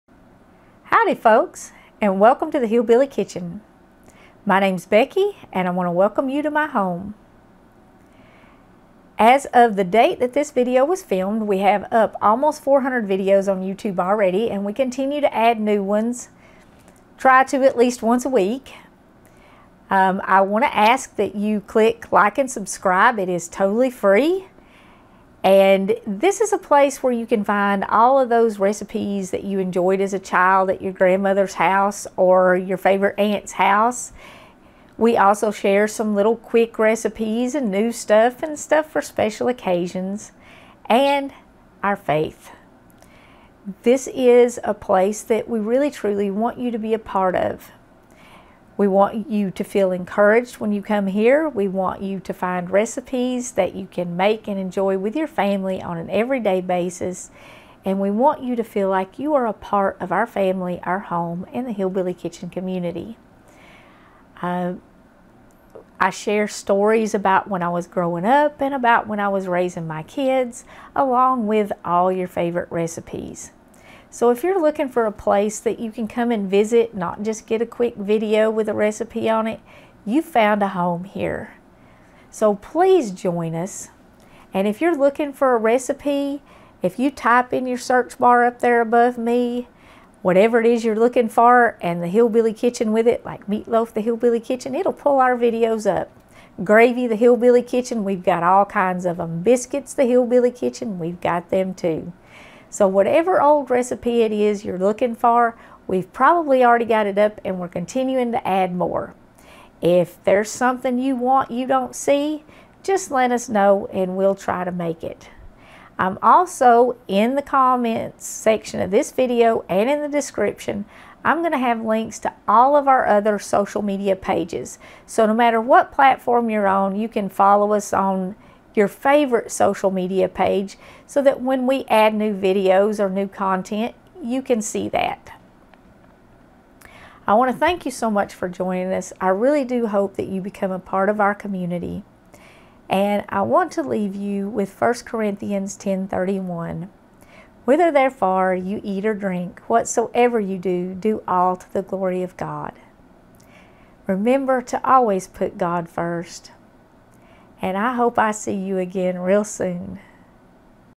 Welcome to The Hillbilly Kitchen
Join Becky, a true country women as she cooks real food in the kitchens where she cooks for her family. She shares her recipes and how to prepare each dish. Food doesn't have to be fancy to taste good. She will teach you how to make basic everyday foods and fancy dishes for special occasions. Along the way she shares her faith and family with you. Cooking at home saves tons of money and is far healthier than eating in restaurants, as a bonus everything is fixed exactly the way you like it.

The kitchen is the heart of the home. Welcome to Becky's kitchen!